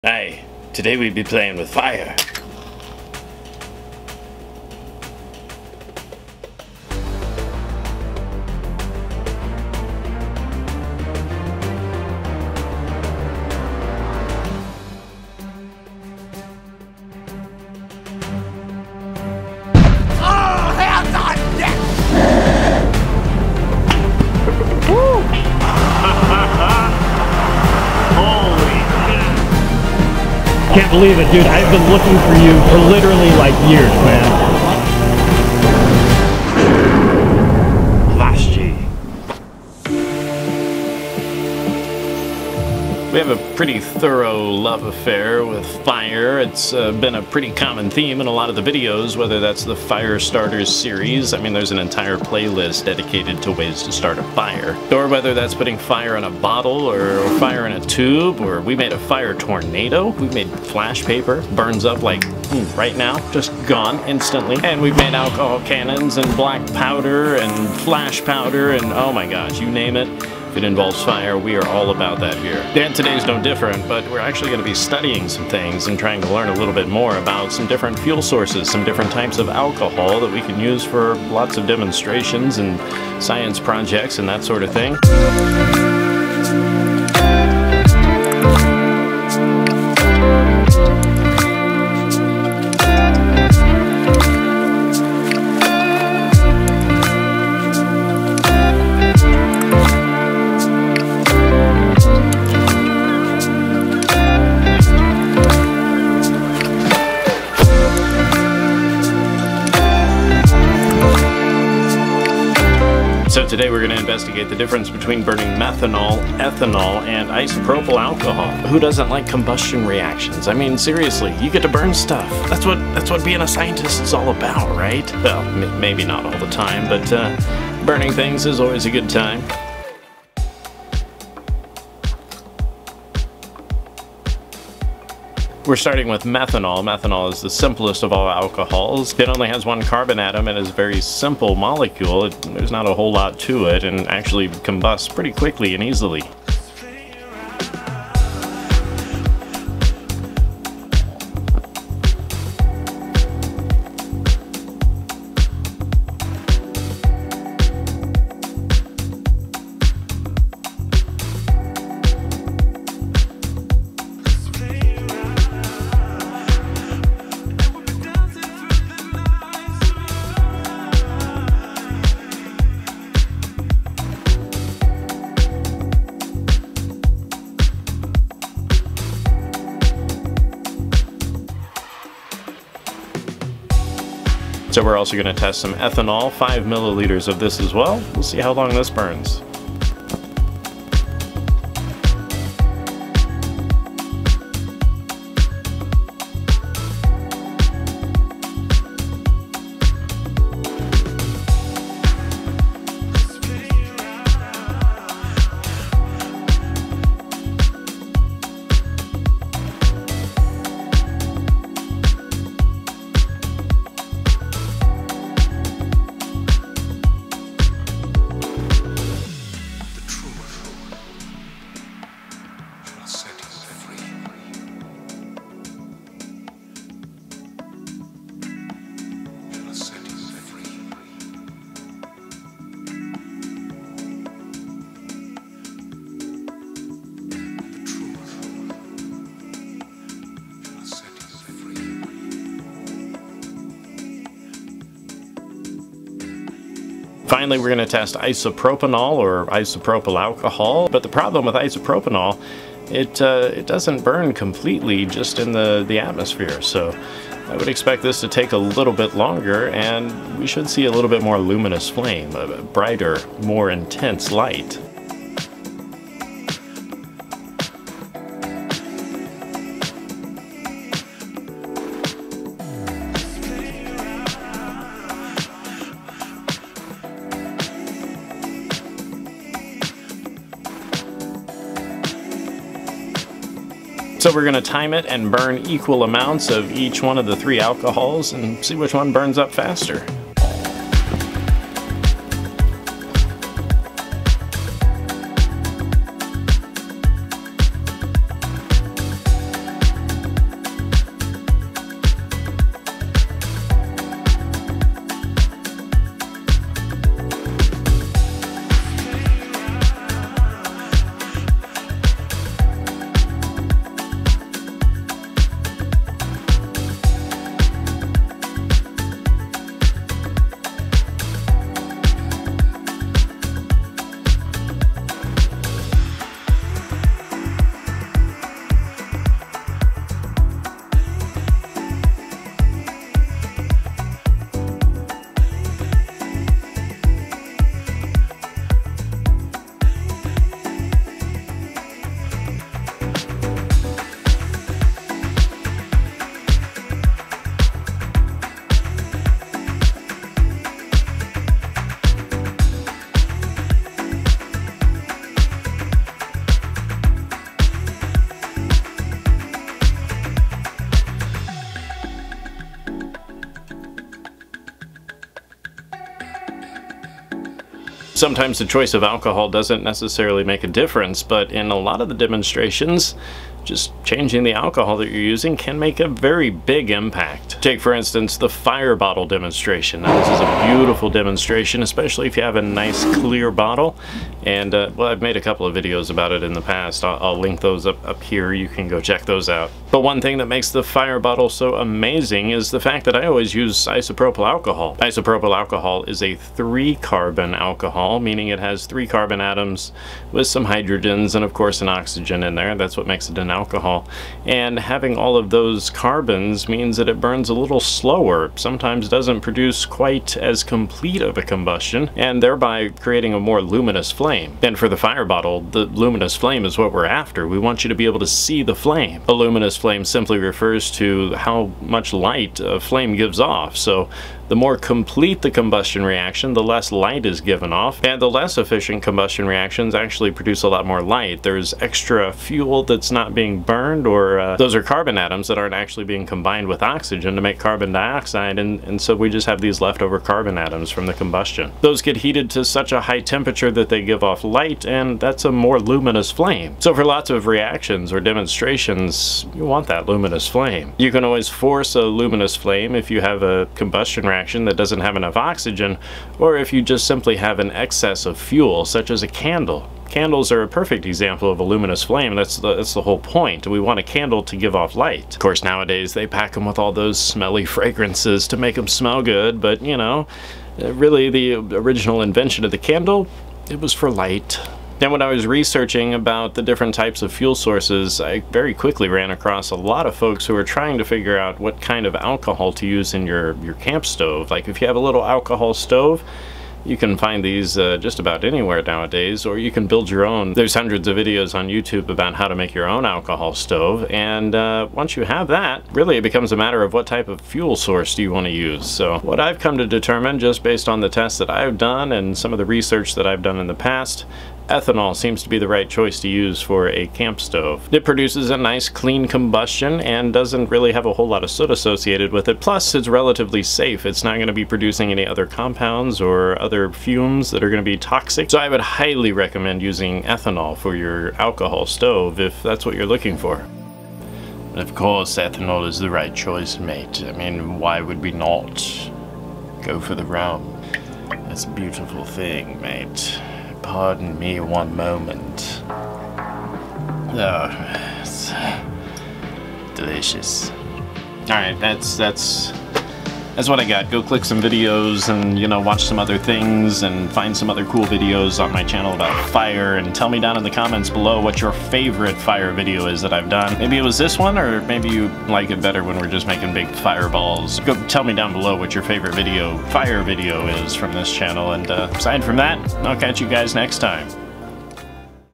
Alcohol Comparison - Methanol vs Ethanol vs Isopropanol
Which alcohol burns better, faster, brighter? I'm comparing methanol, ethanol (grain alcohol) and isopropanol (rubbing alcohol) to see which has a faster rate of combustion, which burns brighter and which is the best choice for an alcohol stove.  A good look at the difference between ethanol and methanol and isopropyl alcohol and a good time with methanol fire, ethanol fire and isopropanol fire! Loads of good information and a great comparison of methanol vs ethanol vs isopropyl alcohol!